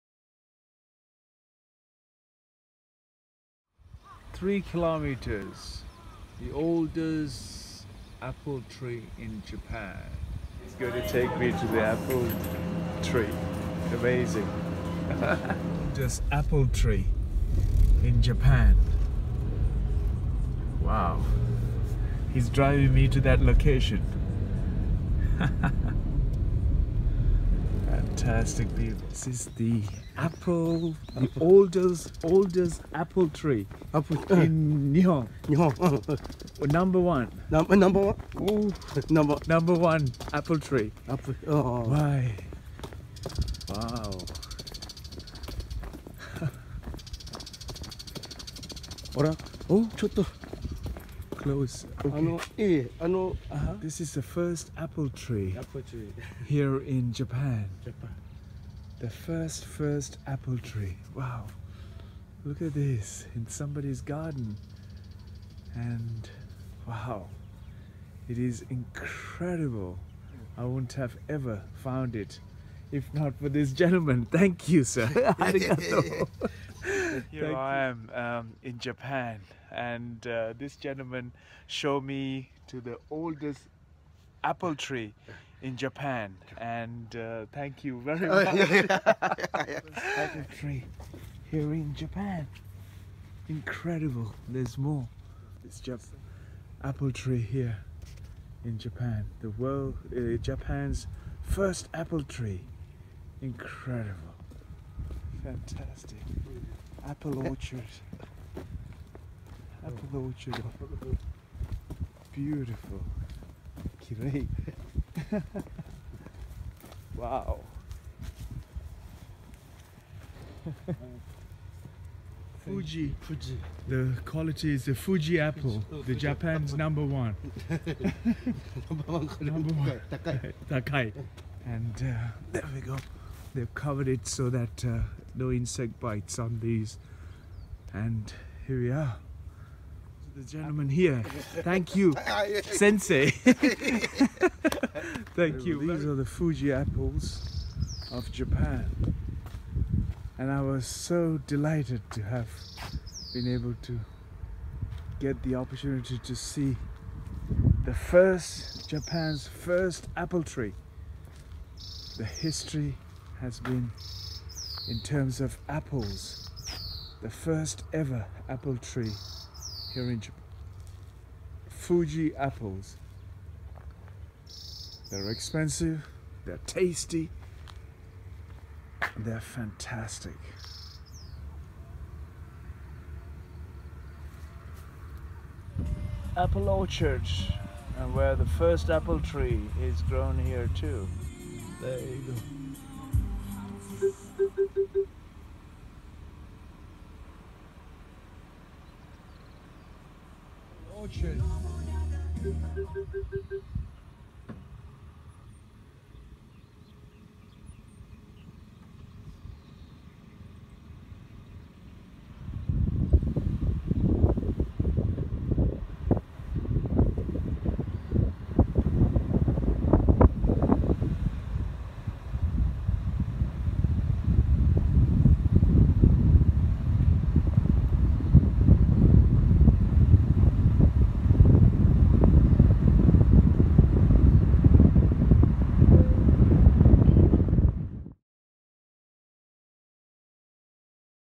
Direction to Oldest Apple Three in Japan
It was a challenge to find the Oldest Apple Tree in Japan. Since the signs show with a arrow.

It then has no direction or signs to the orchard where the apple tree.

There are several Apple Orchards, this very apple tree is in the hidden away farmers gated property.

As I was lost so I stopped at gas station to ask for direction and no one knew where exactly the Tree was.

Luckily you are able to see a local Farmer at the cashier's he was willing was happy to show me the direction to the Apple Tree.

This is only happen in Japan.